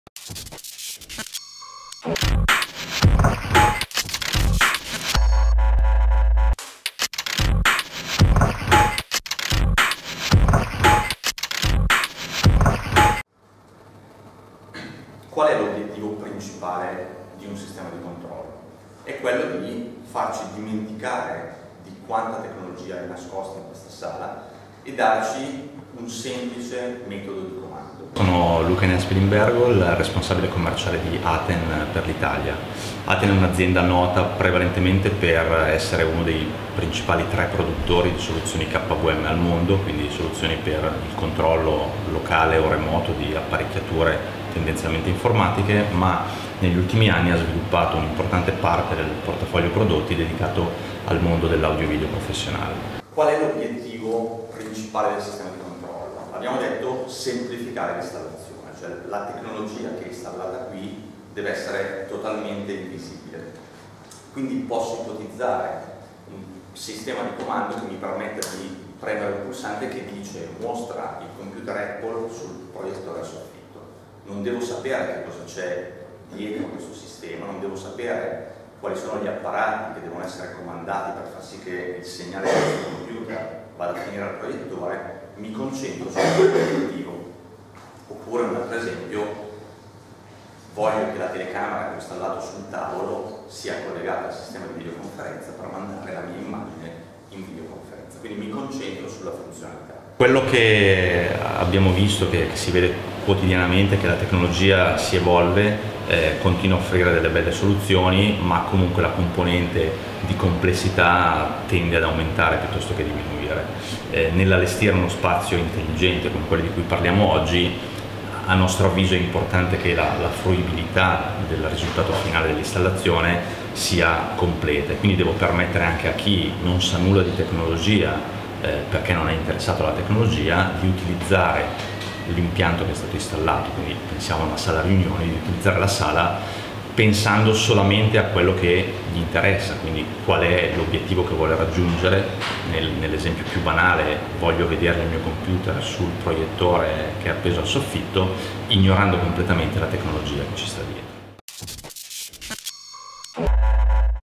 Roadshow Smart Environments, Luca Enea Spilimbergo
Roadshow Smart Environments 2017, Firenze. Intervista a Luca Enea Spilimbergo, Country manager Aten